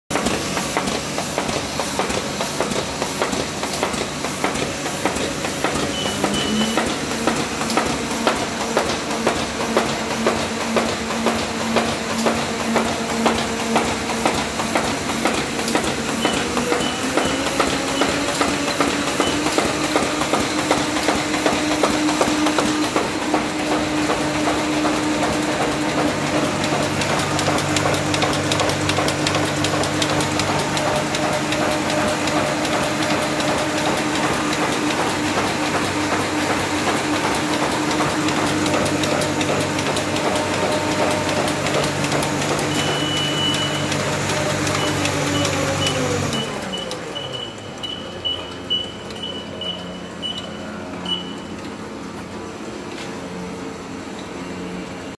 RYOBI 3304HA 2005 | FOR SALE | Bethel Graphics
We can offer Ryobi3304HA
Size 13 × 18*
4 colour offset
Straight press
Model 2003 & 2005 (we have 2 machines)

It was Equipped with
RYOBIMATIC Alhocal Dampening
CPC
Chiller
Pneumatic
Stepdown
SAPC(Auto Plate Change)
Auto Blanket wash
All Chrome cylinders
Max Speed ~ 10,000 Per Hour
Plate Punch
Powder Sprayer
Tools and manuals

It was IMPORTED from France 🇫🇷
Ready STOCK in CHENNAI WAREHOUSE
Test print possible

Bethel Graphics
Chennai ~ 122 | India 🇮🇳